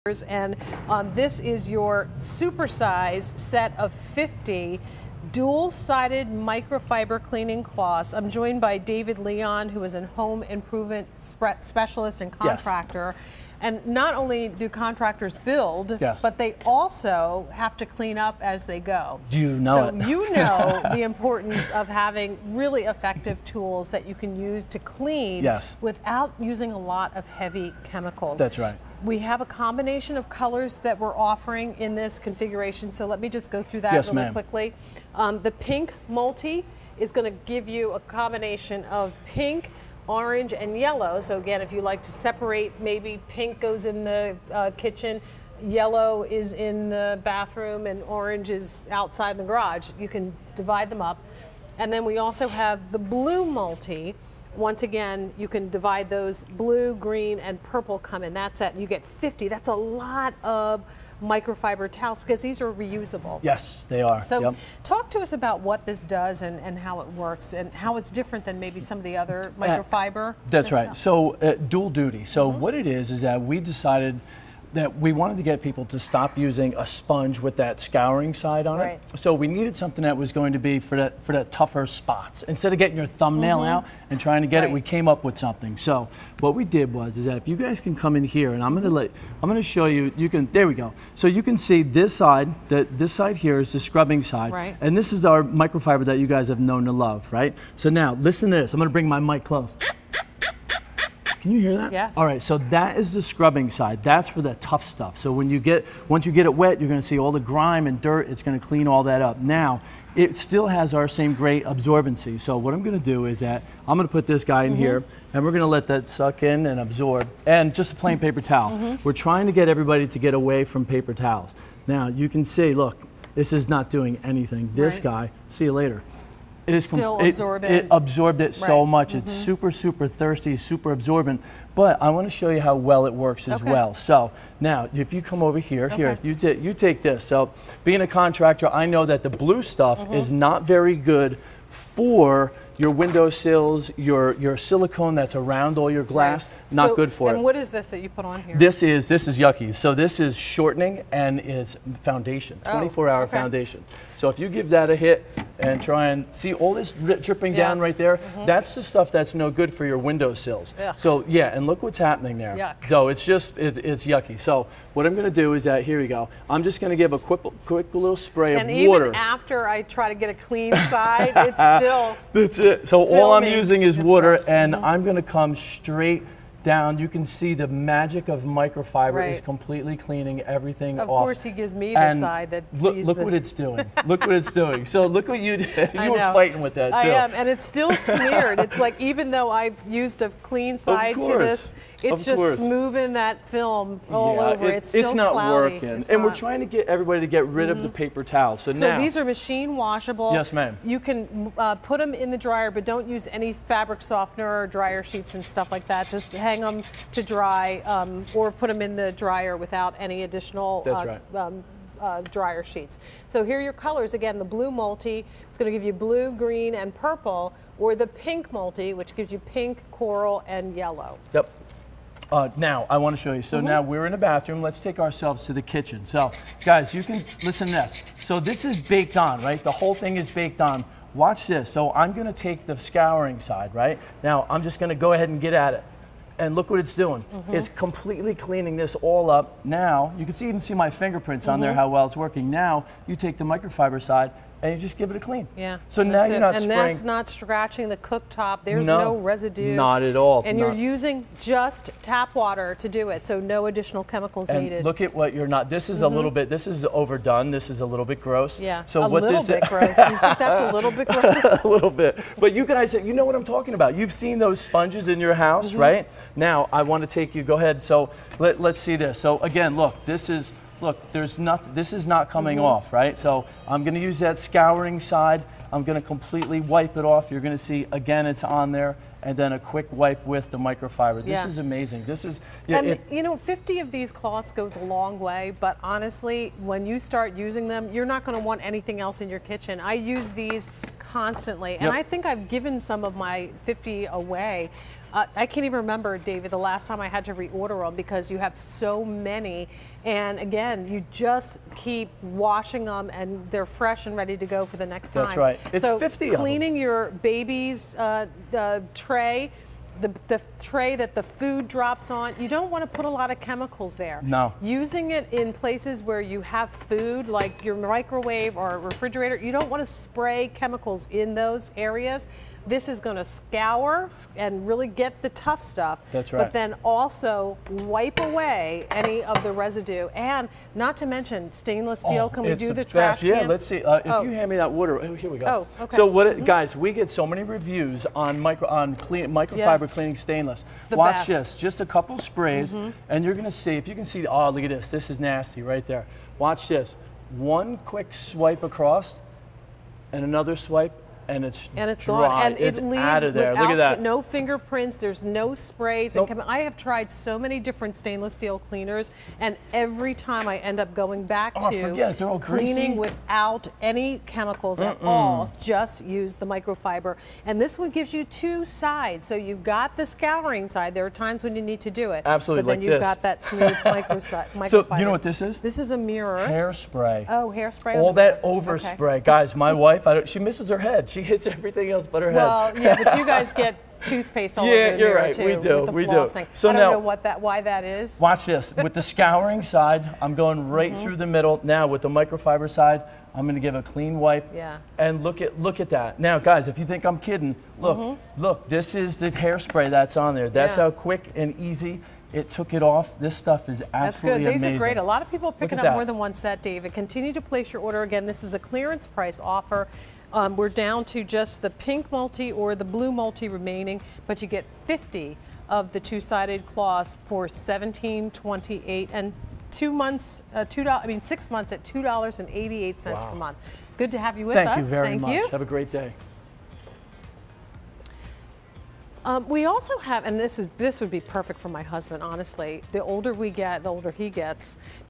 Don Aslett's Super-Size 50-Pc Dual-Side Microfiber Cleaning Cloths on QVC
Don Aslett's Super-Size 50-Pc Dual-Side Microfiber Cleaning Cloths

Say hello to the perfect partner for your daily cleaning needs with this 50-piece set of microfiber cleaning cloths. The dual-sided design gives you options--wipe and dust with the soft side, then flip it over and scrub with the other. From Don Aslett.